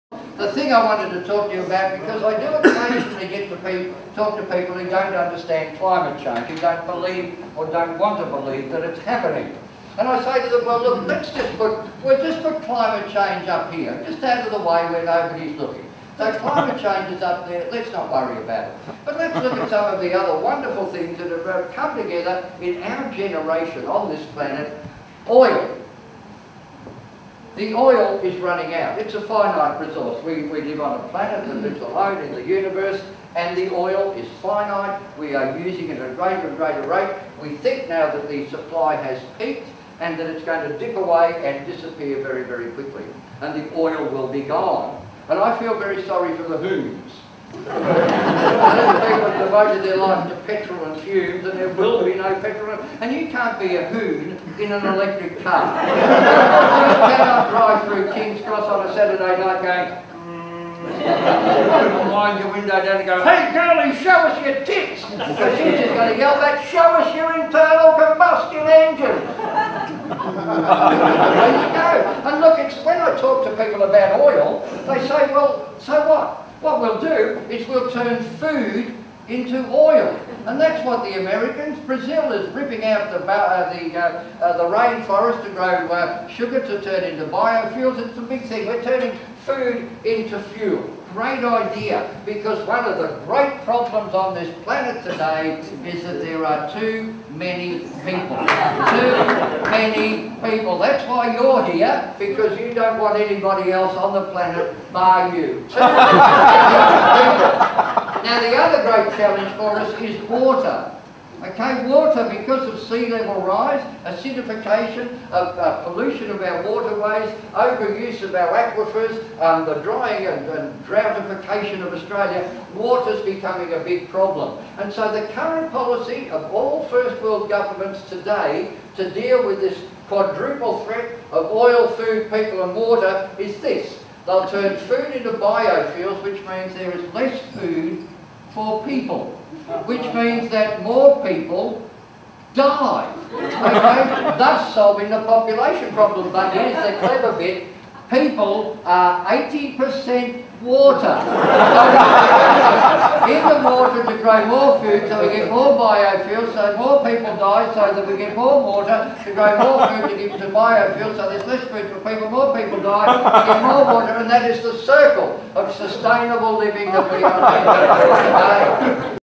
Rod Quantock on Oil, Food, People & Water
Rod expounds on "the circle of sustainable living" at his SPA-sponsored Sydney performance of "Bugger the Polar Bears, This is Serious".